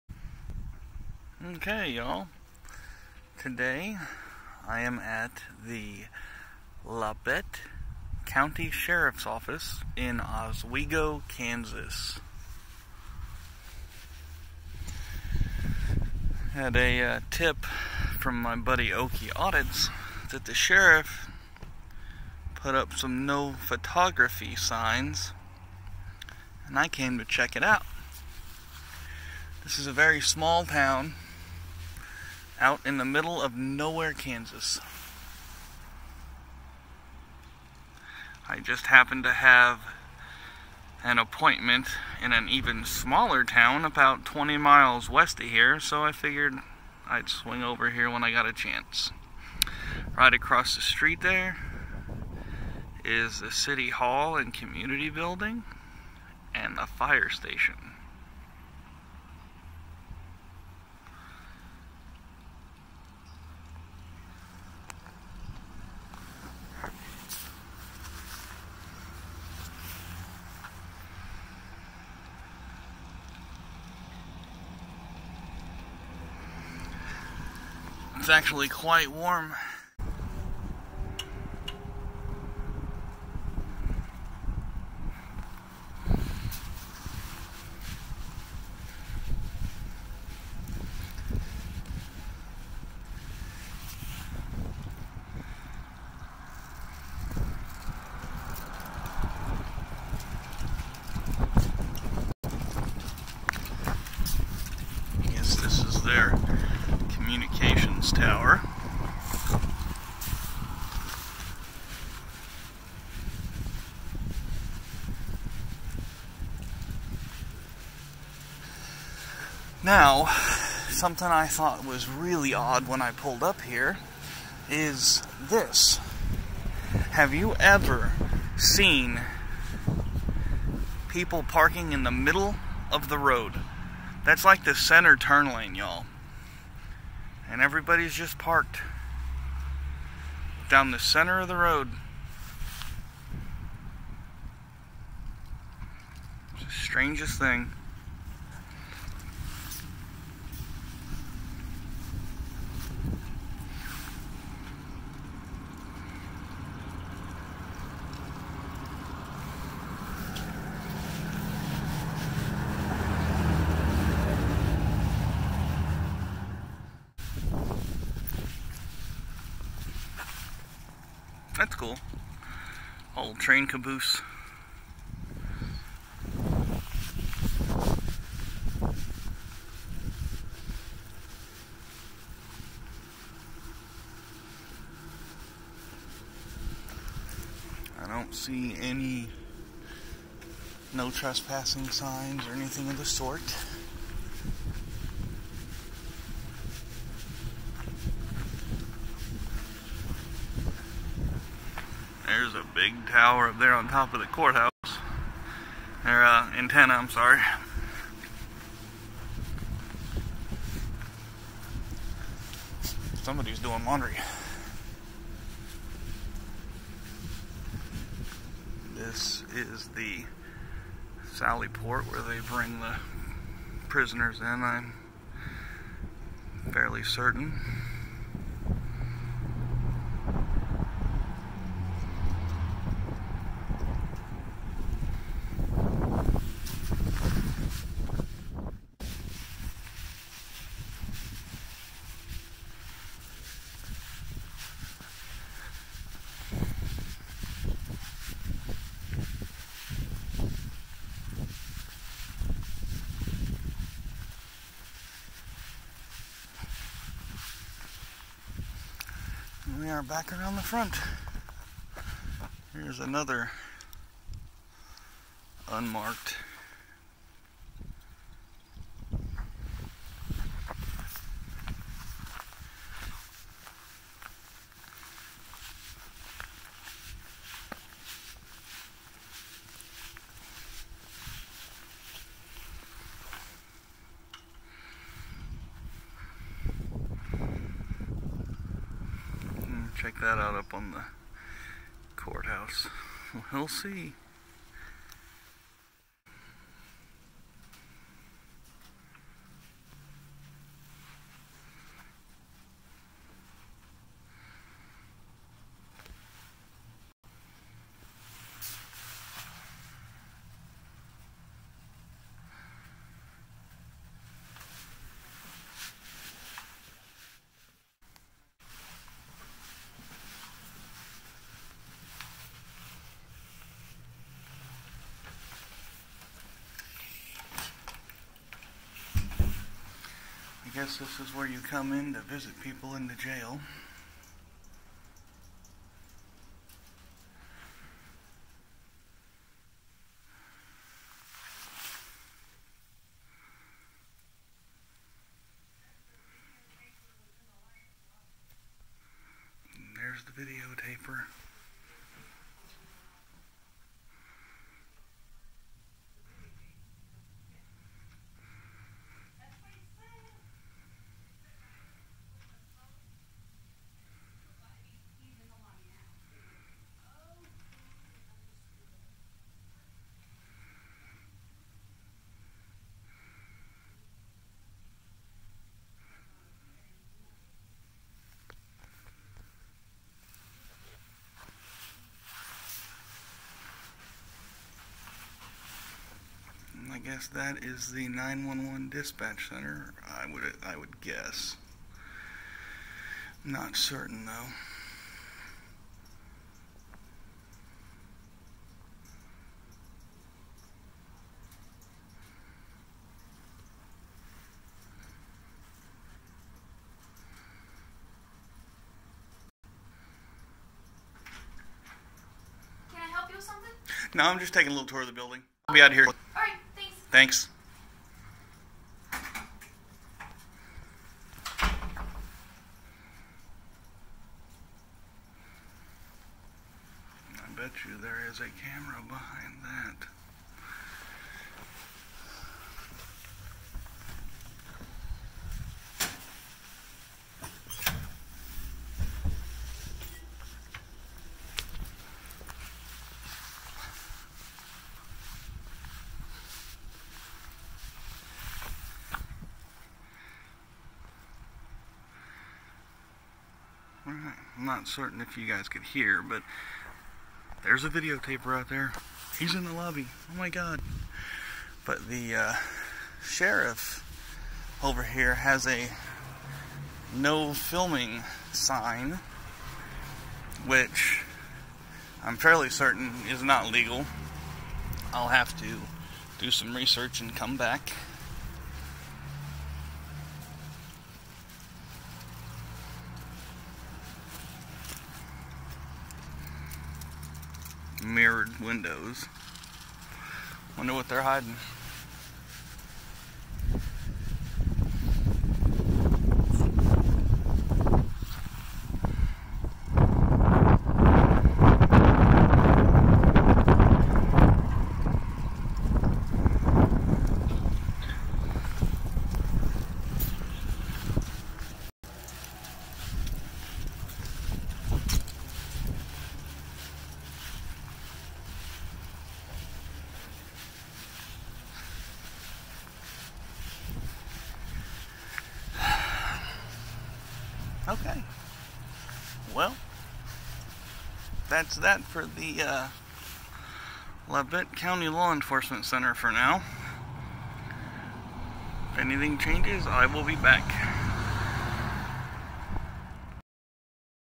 Labette County, Kansas Sheriff Department and 911 Center
This is an audit of the above referenced facility. The sheriff will definitely need a revisit, when I have backup, as a result of his unlawful sign.

Labette County Sheriff
718 5th St
Oswego, Kansas 67356

Please help keep me out here fighting for your rights.